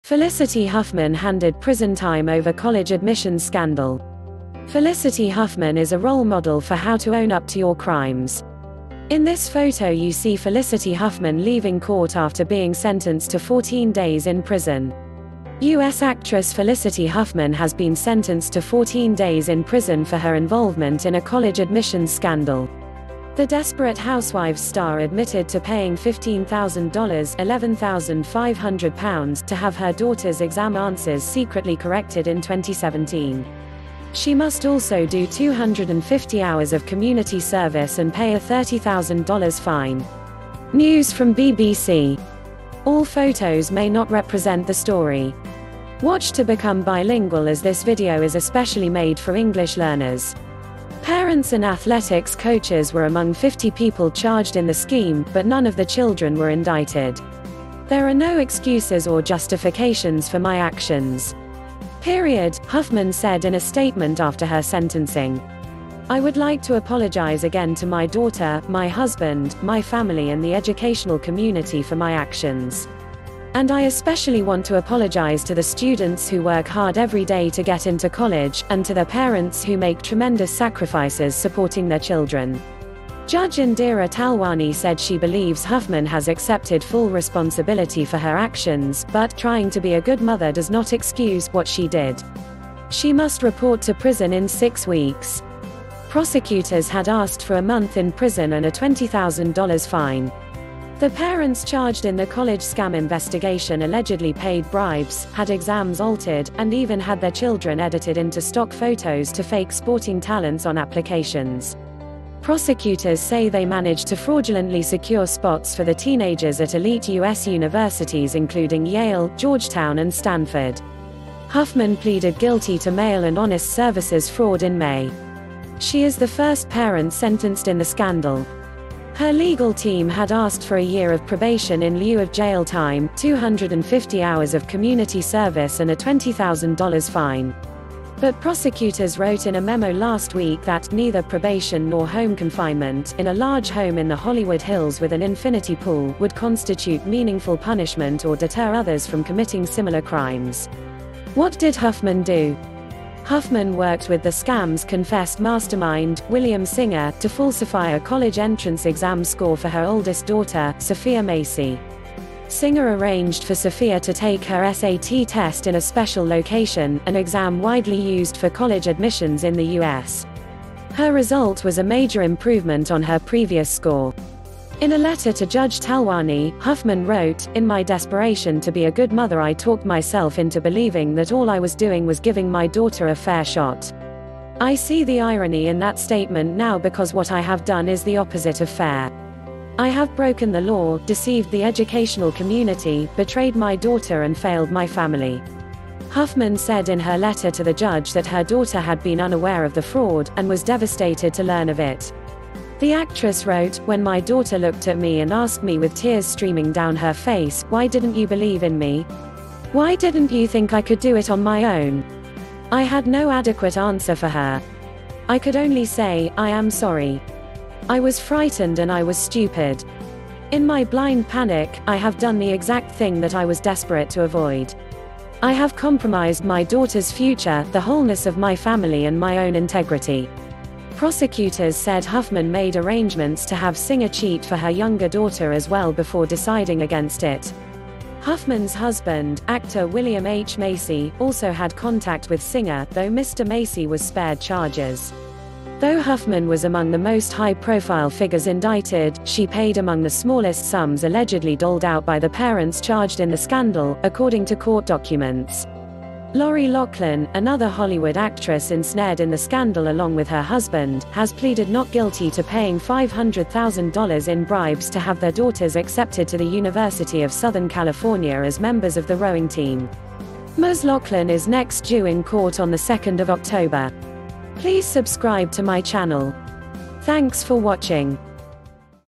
Felicity Huffman handed prison time over college admissions scandal
Felicity Huffman is a role model for how to own up to your crimes.

In this photo you see Felicity Huffman leaving court after being sentenced to 14 days in prison.

US actress Felicity Huffman has been sentenced to 14 days in prison for her involvement in a college admissions scandal.

The Desperate Housewives star admitted to paying $15,000 (£11,500) to have her daughter's exam answers secretly corrected in 2017.

She must also do 250 hours of community service and pay a $30,000 fine.

[News from BBC]

[All Photos may not represent the story.]

[Watch to become bilingual as this video is especially made for English learners.]

Parents and athletics coaches were among 50 people charged in the scheme, but none of the children were indicted.

"There are no excuses or justifications for my actions. Period," Huffman said in a statement after her sentencing. "I would like to apologise again to my daughter, my husband, my family and the educational community for my actions.

"And I especially want to apologise to the students who work hard every day to get into college, and to their parents who make tremendous sacrifices supporting their children."

Judge Indira Talwani said she believes Huffman has accepted full responsibility for her actions, but "trying to be a good mother does not excuse" what she did.

She must report to prison in six weeks. Prosecutors had asked for a month in prison and a $20,000 fine.

The parents charged in the college scam investigation allegedly paid bribes, had exams altered, and even had their children edited into stock photos to fake sporting talents on applications.

Prosecutors say they managed to fraudulently secure spots for the teenagers at elite US universities including Yale, Georgetown and Stanford.

Huffman pleaded guilty to mail and honest services fraud in May. She is the first parent sentenced in the scandal.

Her legal team had asked for a year of probation in lieu of jail time, 250 hours of community service and a $20,000 fine.

But prosecutors wrote in a memo last week that "neither probation nor home confinement (in a large home in the Hollywood Hills with an infinity pool) would constitute meaningful punishment or deter others from committing similar crimes".

What did Huffman do?
Huffman worked with the scam's confessed mastermind, William Singer, to falsify a college entrance exam score for her oldest daughter, Sophia Macy.

Singer arranged for Sophia to take her SAT test in a special location, an exam widely used for college admissions in the US. Her result was a major improvement on her previous score.

In a letter to Judge Talwani, Huffman wrote: "In my desperation to be a good mother I talked myself into believing that all I was doing was giving my daughter a fair shot."

"I see the irony in that statement now because what I have done is the opposite of fair. I have broken the law, deceived the educational community, betrayed my daughter and failed my family."

Huffman said in her letter to the judge that her daughter had been unaware of the fraud, and was devastated to learn of it.

The actress wrote: "When my daughter looked at me and asked me with tears streaming down her face, 'Why didn't you believe in me? Why didn't you think I could do it on my own?' I had no adequate answer for her.

"I could only say, 'I am sorry. I was frightened and I was stupid.' In my blind panic, I have done the exact thing that I was desperate to avoid.

"I have compromised my daughter's future, the wholeness of my family and my own integrity."

Prosecutors said Huffman made arrangements to have Singer cheat for her younger daughter as well before deciding against it.

Huffman's husband - actor William H Macy - also had contact with Singer, though Mr Macy was spared charges.

Though Huffman was among the most high-profile figures indicted, she paid among the smallest sums allegedly doled out by the parents charged in the scandal, according to court documents.

Lori Loughlin, another Hollywood actress ensnared in the scandal along with her husband, has pleaded not guilty to paying $500,000 in bribes to have their daughters accepted to the University of Southern California as members of the rowing team.

Ms Loughlin is next due in court on 2 October.